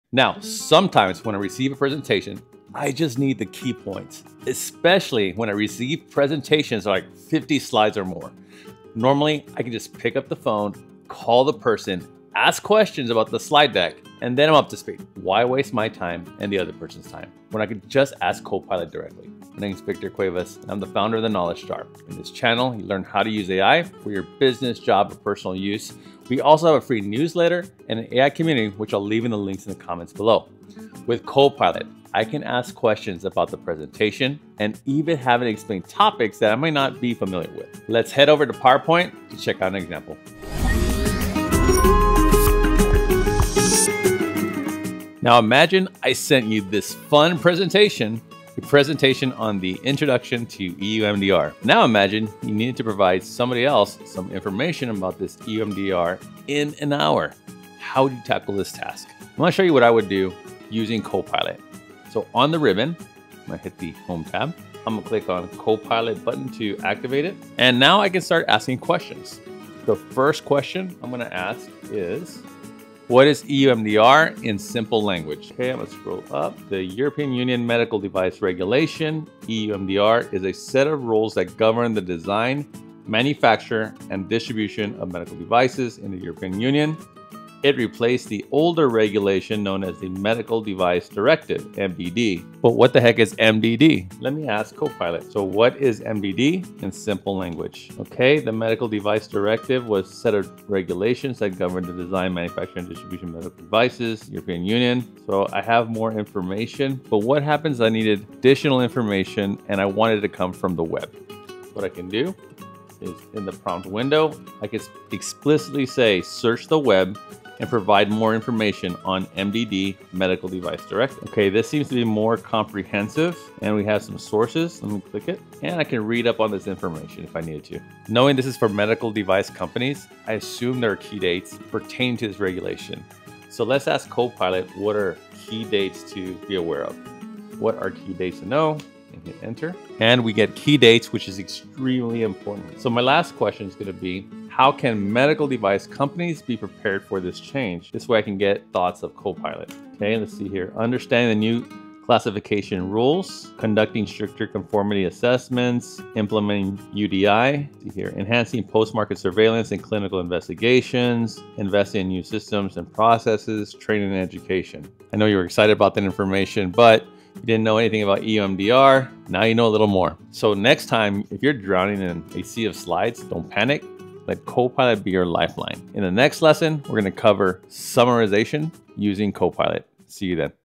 Can Copilot REALLY Help You Ask Smarter Questions in PowerPoint?
Unlock the full potential of PowerPoint with Copilot! In this video, we'll explore how this innovative tool can revolutionize the way you ask questions and interact with your audience. From generating smarter questions to streamlining your presentation workflow, we'll dive into the capabilities of Copilot and show you how it can take your PowerPoint skills to the next level. Whether you're a student, teacher, or business professional, this video is perfect for anyone looking to get more out of PowerPoint and make a lasting impression.

Resources
🛠️ Free AI Community:

Work with The Knowledge Jar
🤝Course Creation, AI Solutions, Websites, Mobile Applications
       / victorcuevasii